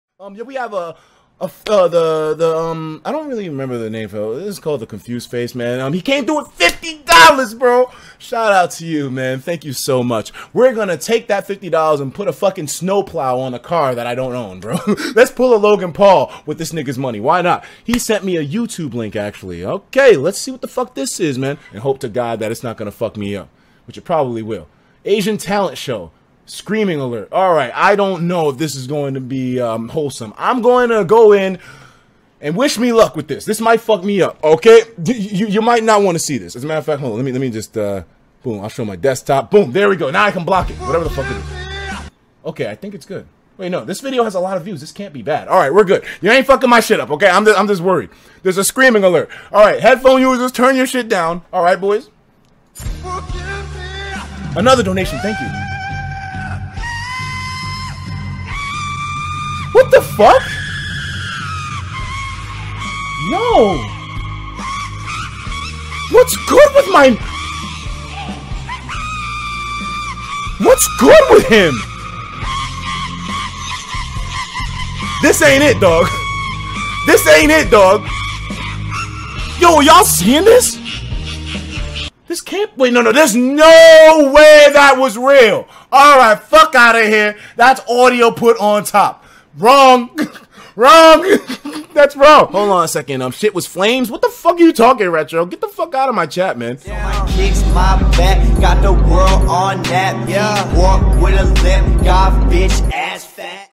ETIKA REACTS TO ASIAN TALENT SHOW - (SCREAMING ALERT)
ETIKA REACTS TO KPOP DUDE SINGING BADLY ( MEME )